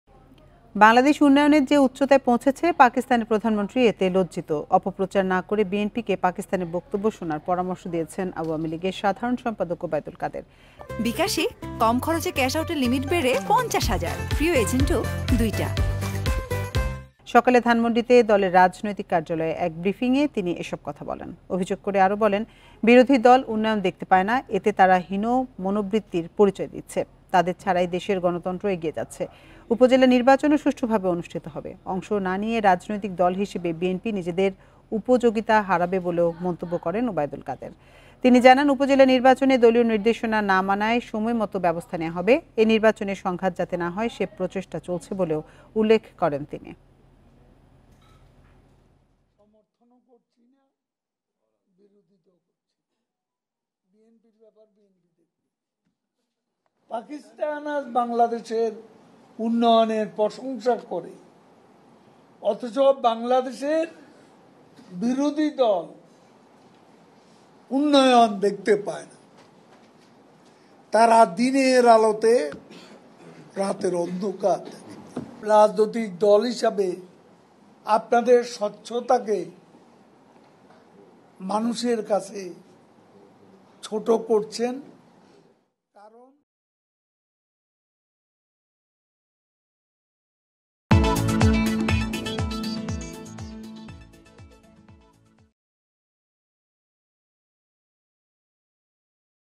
বাংলাদেশ উন্নয়নের যে উচ্চতায় পৌঁছেছে পাকিস্তানের প্রধানমন্ত্রী এতে লজ্জিত: কাদের | Independent TV
বাংলাদেশ উন্নয়নের যে উচ্চতায় পৌঁছেছে পাকিস্তানের প্রধানমন্ত্রী এতে লজ্জিত। অপপ্রচার না করে বিএনপিকে পাকিস্তানের বক্তব্য শোনার পরামর্শ দিয়েছেন আওয়ামী লীগের সাধারণ সম্পাদক ওবায়দুল কাদের।
The Prime Minister of Pakistan is ashamed of the height that Bangladesh has reached. Awami League General Secretary Obaidul Quader advised BNP to listen to Pakistan's statement without spreading false propaganda.